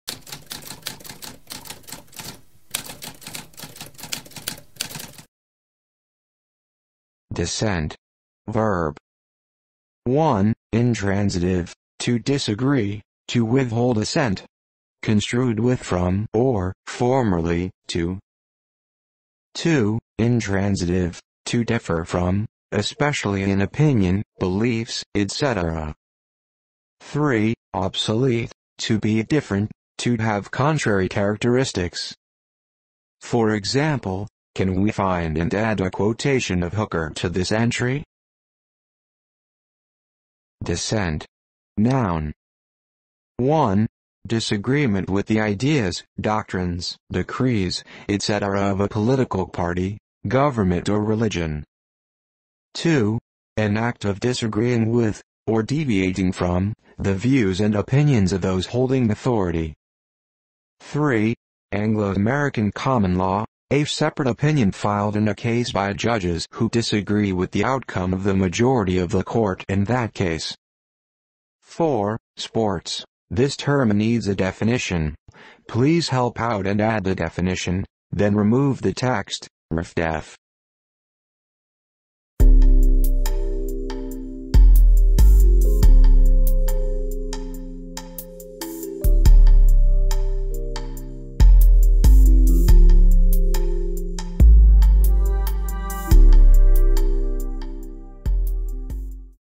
What does dissent mean?
A spoken definition of dissent.

Intro Sound:
Typewriter - Tamskp